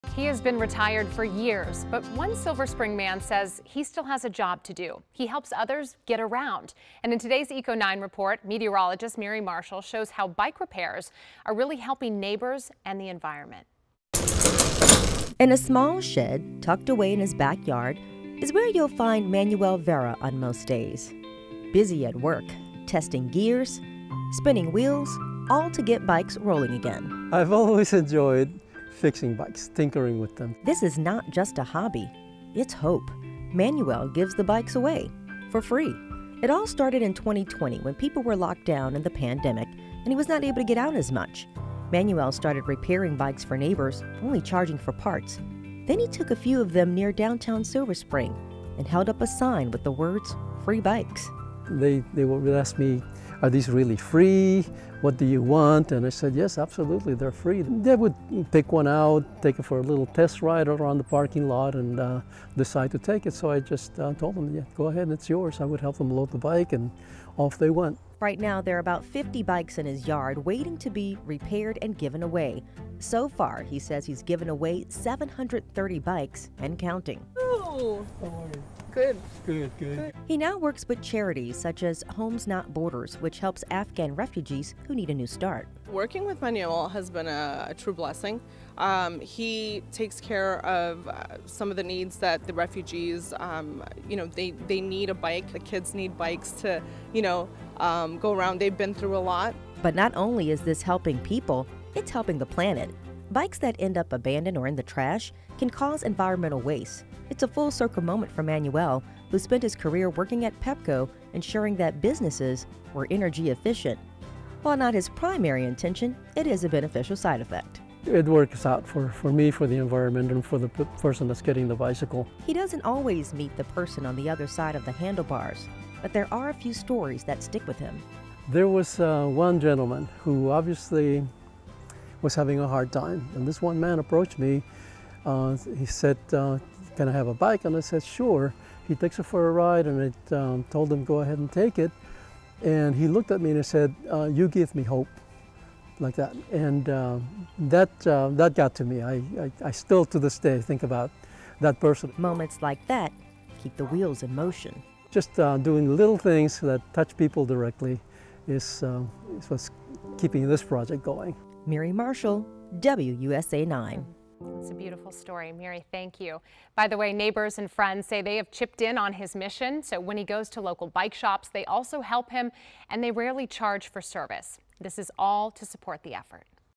Meet the Silver Spring man who has given away more than 700 used bikes for free
Manuel Vera is a good Samaritan impacting his community: keeping bikes out of landfills to help environment and people.